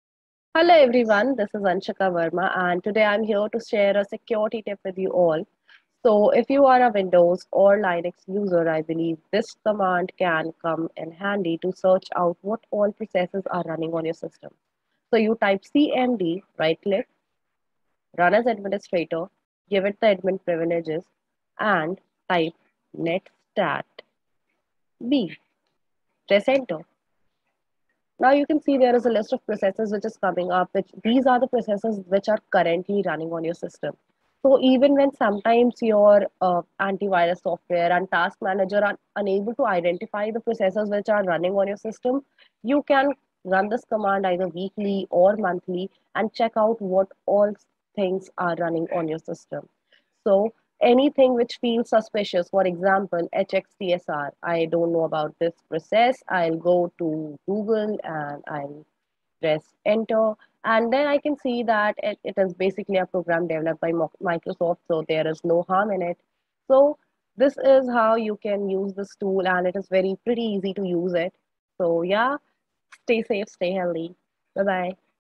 Some Tips by Anshika Verma Listen to her in Cyber Security Awareness Month 2021 | Kleap Con
Watch the video of Ayushi Verma some cyber security tips in Cyber Security Awareness Month 2021.